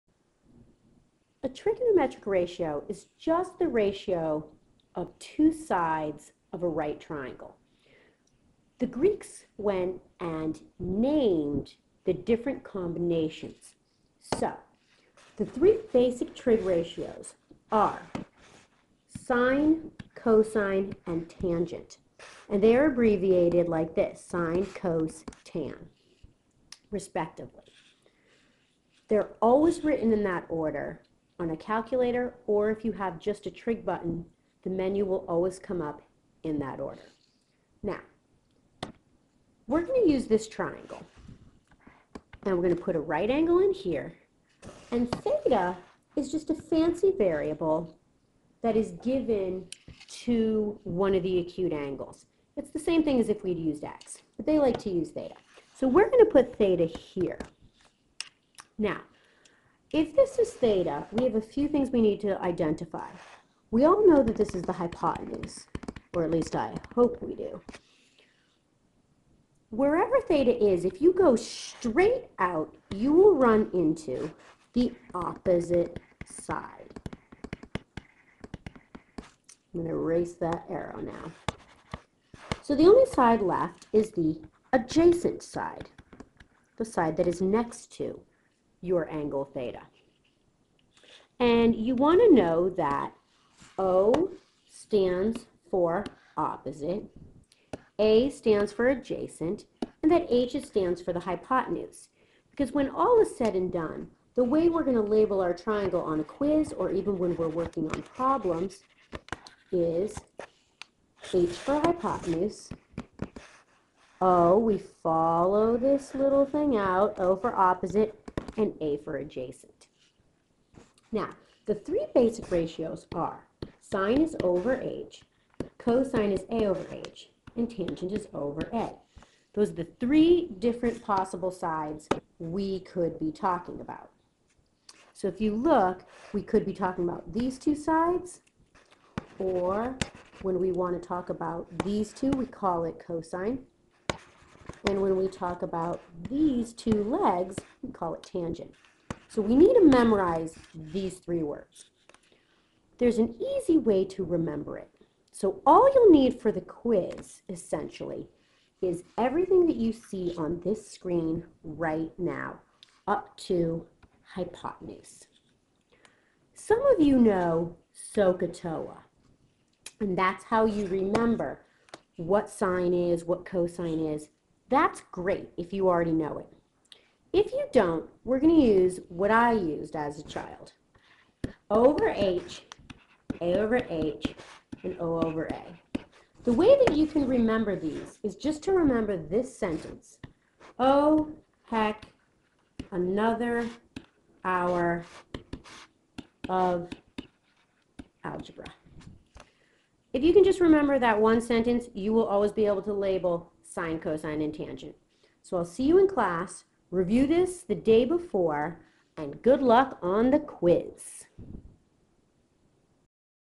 trig ratios Vnotes
basic notes on what trig ratios are - no info on how to solve for missing sides/angles